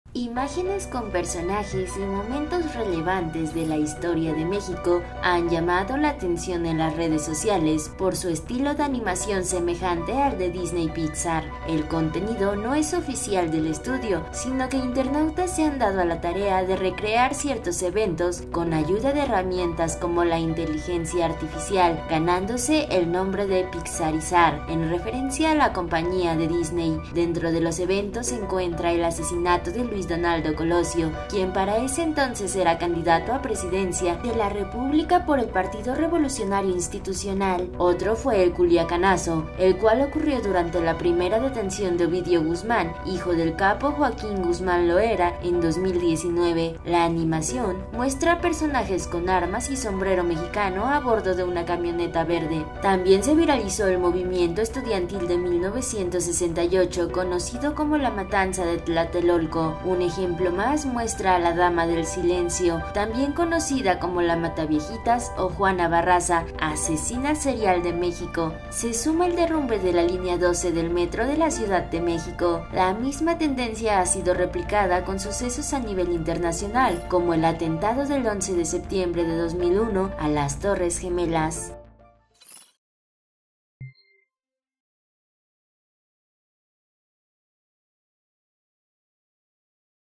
"Pixarizan" episodios históricos de México en redes sociales, entre ellos Colosio
La tendencia “pixarizar” consiste en recrear personajes o escenarios semejantes a los del estudio de animación propiedad de The Walt Disney Company